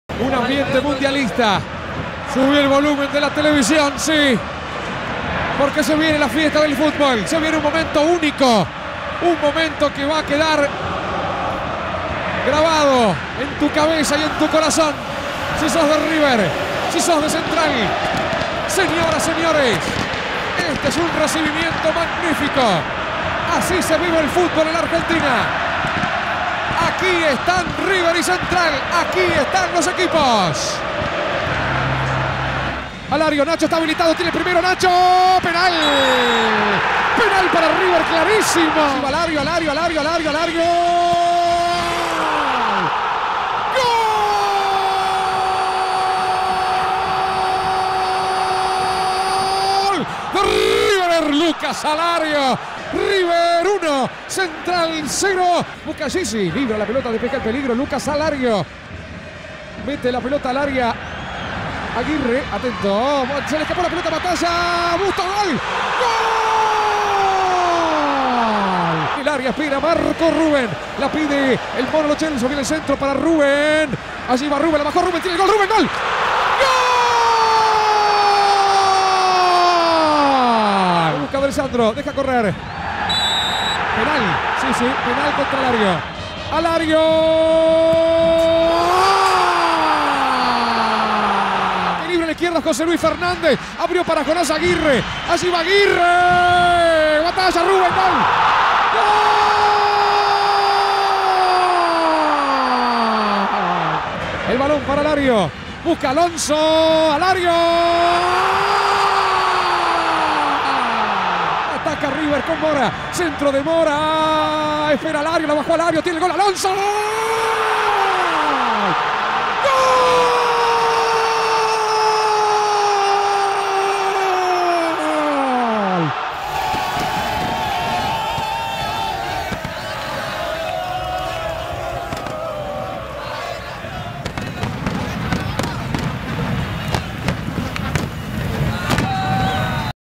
EfeméridesCopaArgentina: River - Rosario Central (Final de la edición 2015-2016)
Goles y resultado cambiante para una de las definiciones más emotivas